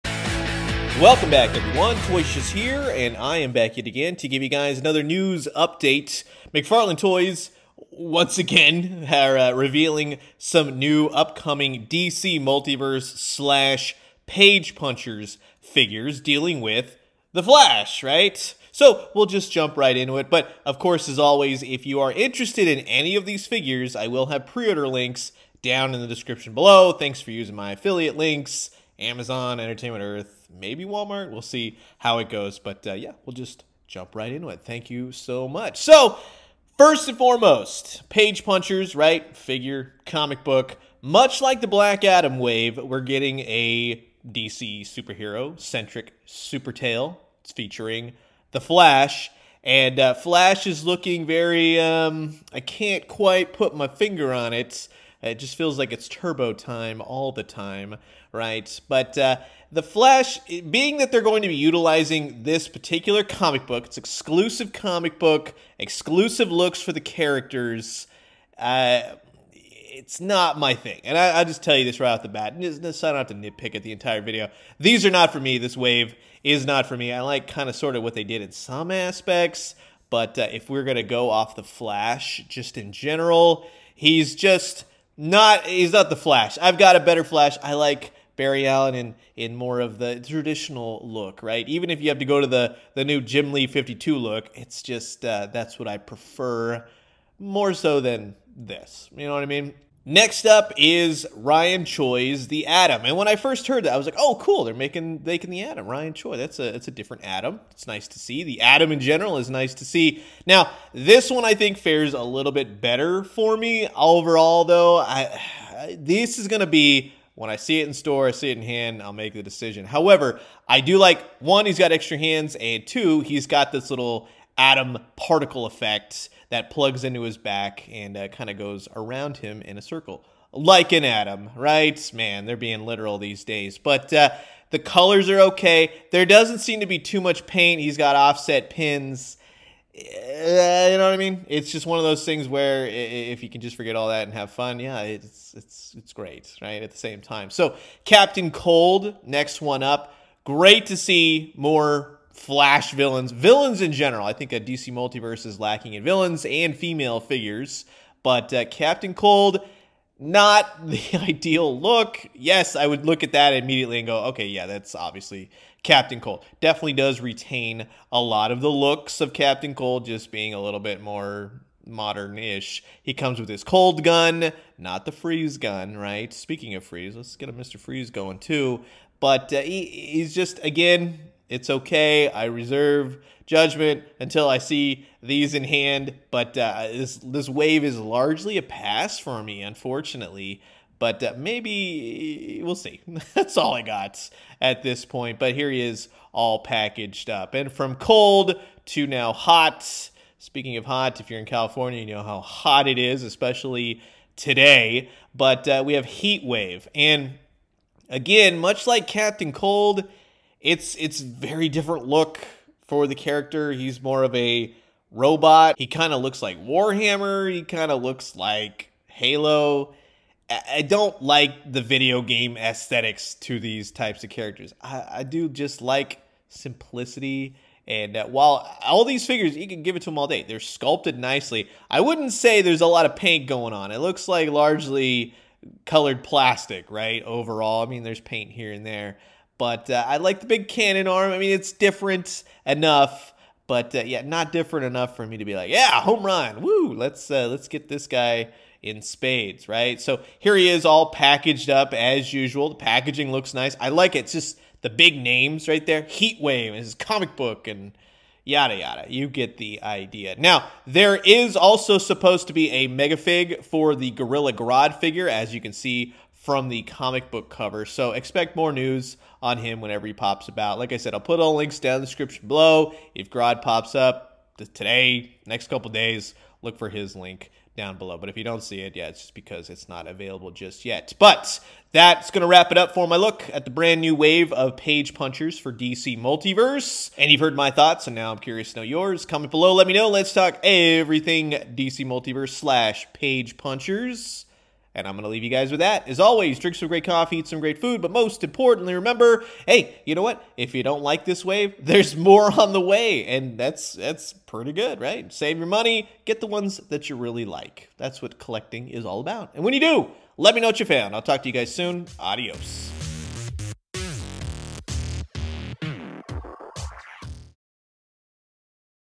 2022 NEW 7in FLASH PAGE PUNCHERS WAVE REVEALED | McFarlane Toys
Everything you need to know about the brand new wave announced by McFarlane Toys, 7in PAGE PUNCHERS ft. THE FLASH!

Thanks in advance for using my links, they’ll help the channel to grow!

Gorilla Grodd Mega Figure

The Flash

The Atom

Captain Cold